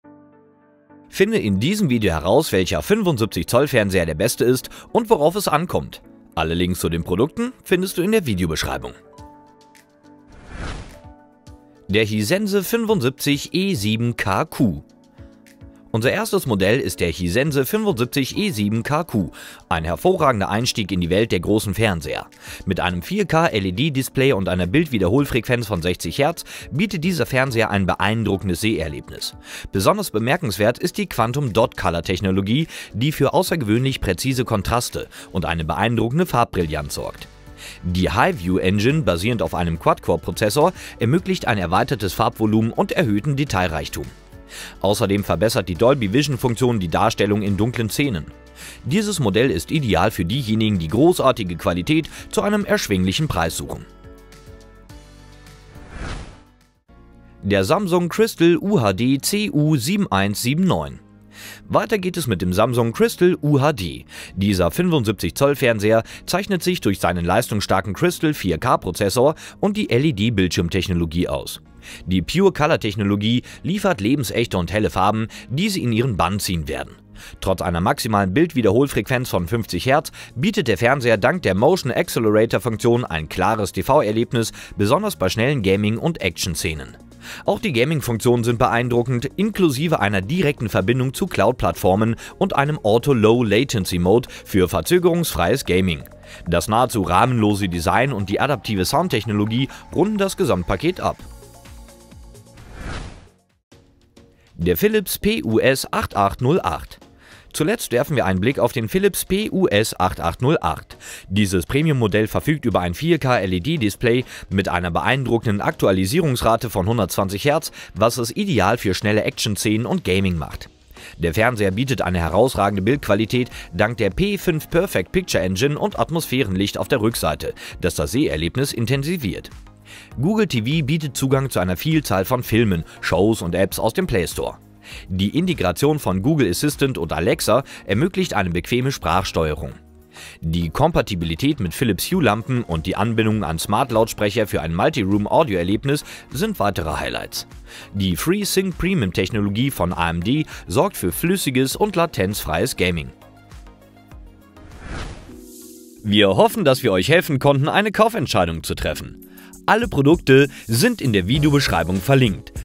Die besten 75 Zoll Fernseher (2025) - TESTSIEGER im Vergleich!
Bester 75 Zoll Fernseher im Test bzw. Vergleich 2025
★ Vergleichssieger ★
► Samsung CU7179

★ Preis-Leistungs-Sieger ★
► Hisense 75E7KQ

★ Beste Bewertungen ★
► Philips PUS 8808

⏱🚨🚛 ⎮ 24H LIEFERUNG!

Welcher 75 Zoll Fernseher ist der Beste? In unserem Video zeigen wir euch die besten 75 Zoll Fernseher im Test bzw. Vergleich, welche es nach unserer Empfehlung auf Amazon zu kaufen gibt.

Wir haben unterschiedliche Hersteller verglichen, darunter der 75 Zoll Fernseher von Samsung, ein 75 Zoll Fernseher von Hisense und der 75 Zoll Fernseher von Philips.

Liebe Grüße
Dein Vergleichssieger-Team

0:00​ Intro
0:11 Hisense 75E7KQ 75 Zoll Fernseher
0:56 Samsung CU7179 75 Zoll Fernseher
1:43 Philips PUS 8808 75 Zoll Fernseher